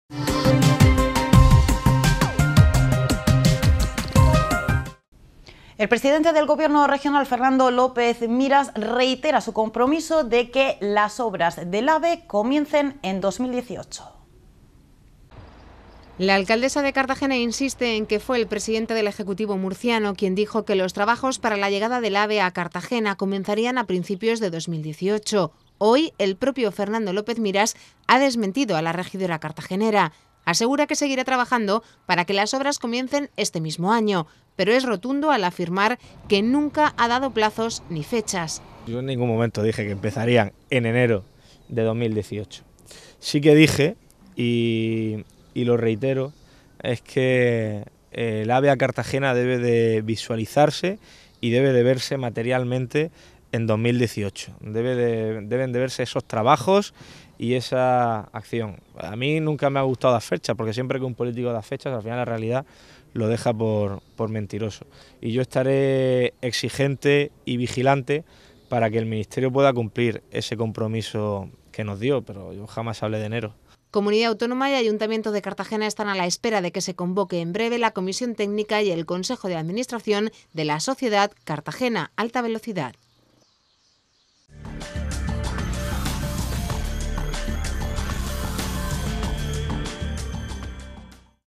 Llegada del AVE a Cartagena
El Presidente del ejecutivo murciano niega que le dijera a la alcaldesa que las obras para la llegada del AVE a Cartagena fueran a comenzar en enero. Fernando López Miras sí mantiene su compromiso de que los trabajos se inicien en 2018